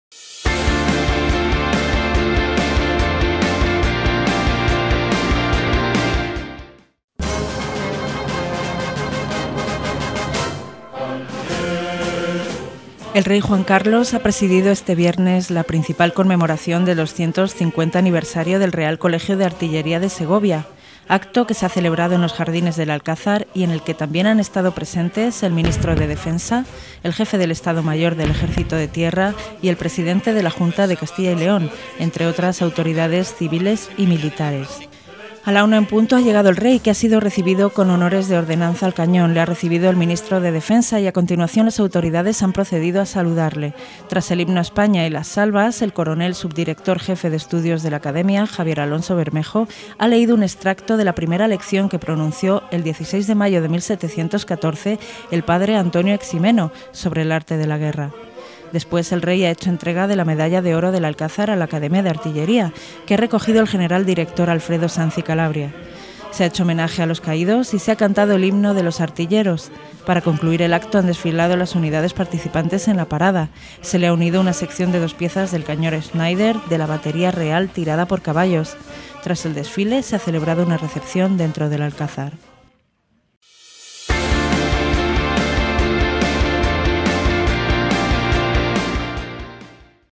El Rey preside el Acto principal del 250º Aniversario del Real Colegio de Artillería de Segovia
El Rey Juan Carlos ha presidido este viernes la principal conmemoración del 250º Aniversario del Real Colegio de Artillería de Segovia. Acto que se ha celebrado en los jardines del Alcázar y en el que también han estado presentes el ministro de Defensa, el jefe del Estado Mayor del Ejército de Tierra y el presidente de la Junta de Castilla y León, entre otras autoridades civiles y militares.